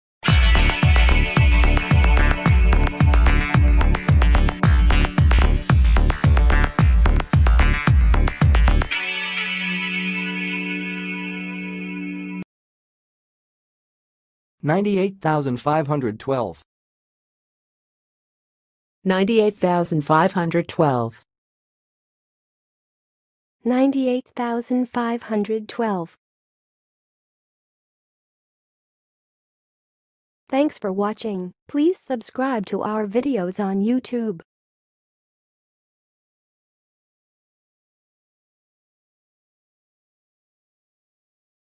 98512 - HOW TO PRONOUNCE IT?
98512 - Don't mispronounce 98512 anymore in 2023

The input topic 98512 does not provide any specific context.
It appears to be a random sequence of numbers with no inherent meaning.

Pronunciation:
Since 98512 is a numerical sequence, it should be pronounced as 'ninety-eight thousand five hundred twelve'.
Alternatively, it can be pronounced as individual digits: 'nine eight five one two' or 'nine eighty-five twelve' depending on the intended usage.